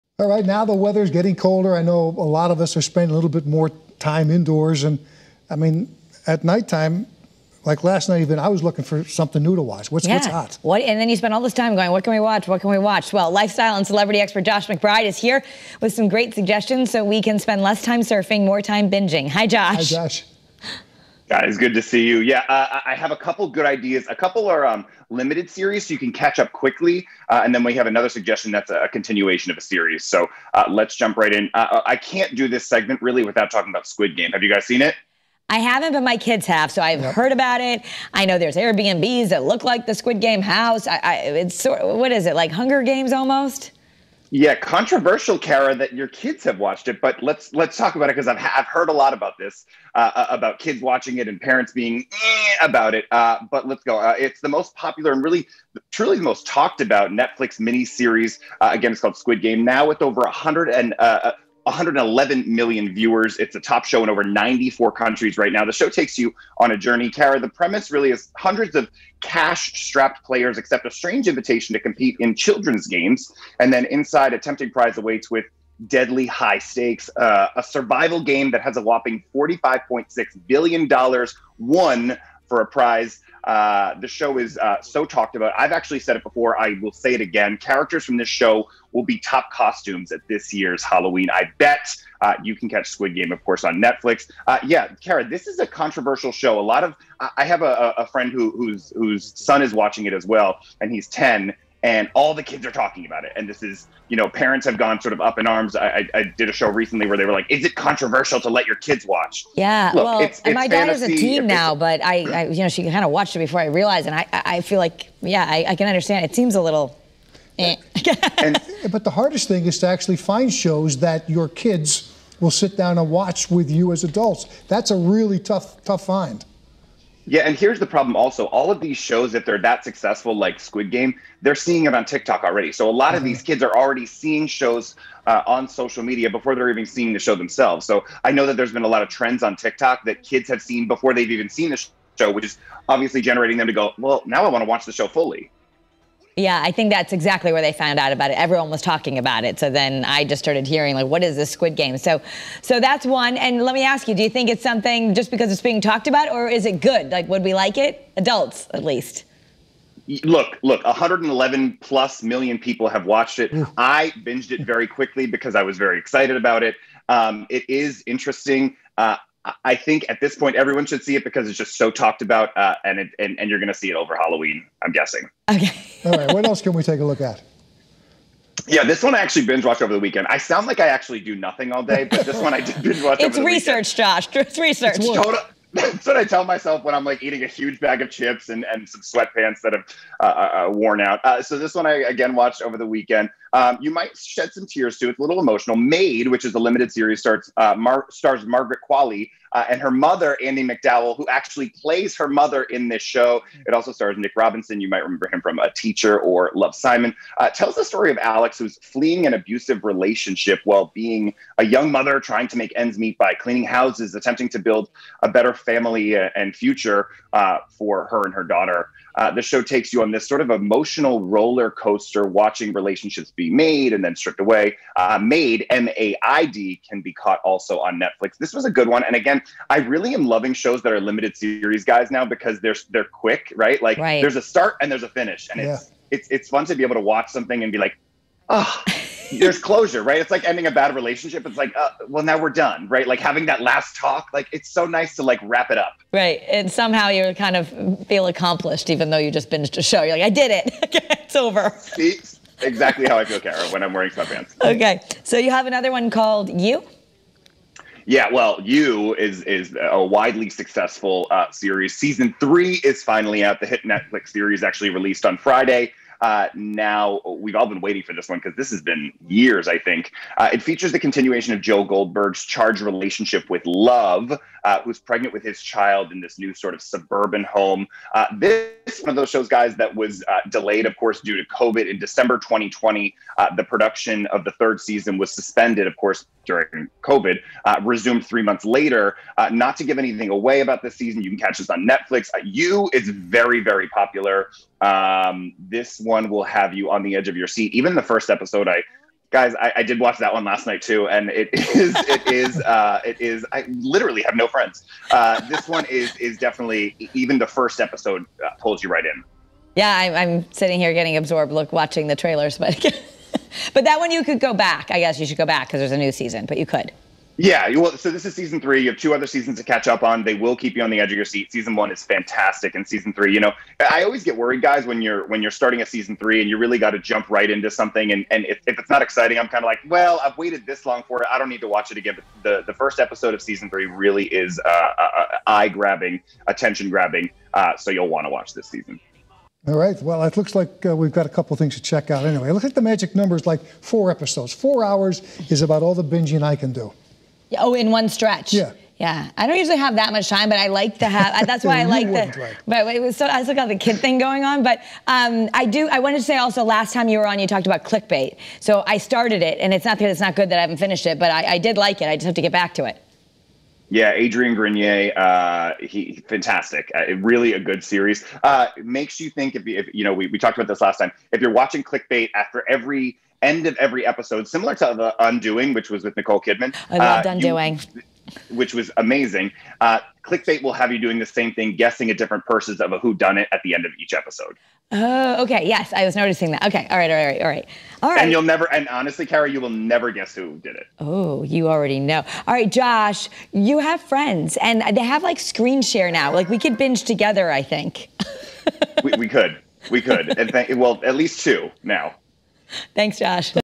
New binge-worthy shows
Lifestyle and celebrity expert Josh McBride talks about binge-worthy shows you can stream.